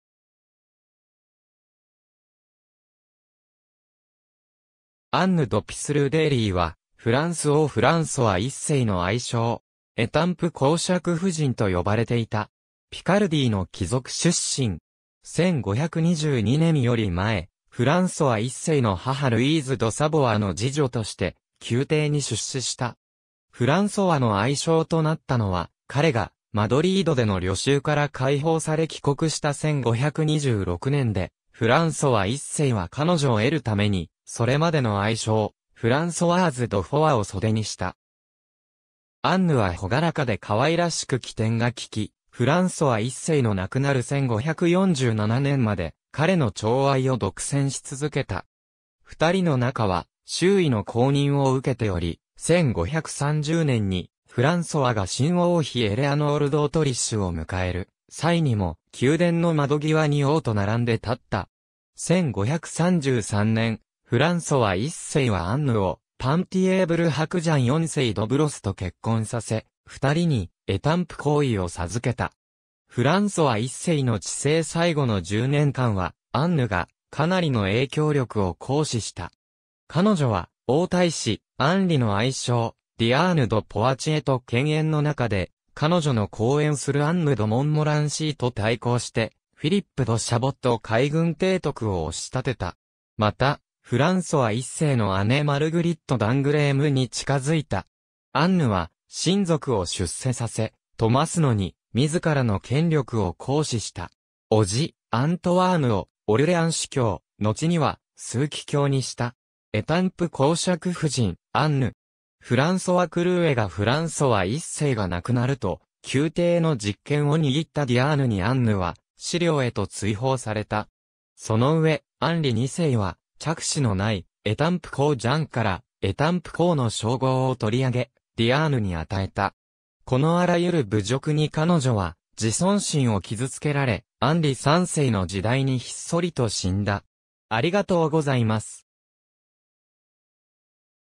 アンヌ・ド・ピスルー・デイリー
アンヌ・ド・ピスルー・デイリー（Anne de Pisseleu d'Heilly, 1508年 - 1580年）は、フランス王フランソワ1世の愛妾。
エタンプ公爵夫人（Duchesse d'Etampes）と呼ばれていた。
ピカルディーの貴族出身。
1522年より前、フランソワ1世の母ルイーズ・ド・サヴォワの侍女として、宮廷に出仕した。
フランソワの愛妾となったのは、彼がマドリードでの虜囚から解放され帰国した1526年で、フランソワ1世は彼女を得るためにそれまでの愛妾フランソワーズ・ド・フォワを袖にした。
アンヌは朗らかで可愛らしく機転がきき、フランソワ1世の亡くなる1547年まで彼の寵愛を独占し続けた。
2人の仲は周囲の公認を受けており、1530年にフランソワが新王妃エレアノール・ドートリッシュ（神聖ローマ皇帝カール5世の姉）を迎える際にも、宮殿の窓際に王と並んで立った。
1533年、フランソワ1世はアンヌをパンティエーヴル伯ジャン4世・ド・ブロスと結婚させ、2人にエタンプ公位を授けた。
フランソワ1世の治世最後の10年間は、アンヌがかなりの影響力を行使した。
彼女は王太子アンリ（のちのアンリ2世）の愛妾ディアーヌ・ド・ポワチエと犬猿の仲で、彼女の後援するアンヌ・ド・モンモランシーと対抗して、フィリップ・ド・シャボット海軍提督を押し立てた。
また、フランソワ1世の姉マルグリット・ダングレームに近づいた。
アンヌは、親族を出世させ富ますのに自らの権力を行使した。
叔父アントワーヌをオルレアン司教、のちには枢機卿にした。
エタンプ公爵夫人アンヌ。
フランソワ・クルーエ画 フランソワ1世が亡くなると、宮廷の実権を握ったディアーヌにアンヌは私領へと追放された。
そのうえ、アンリ2世は嫡子のないエタンプ公ジャンからエタンプ公の称号を取り上げ、ディアーヌに与えた。
このあらゆる侮辱に彼女は自尊心を傷つけられ、アンリ3世の時代にひっそりと死んだ。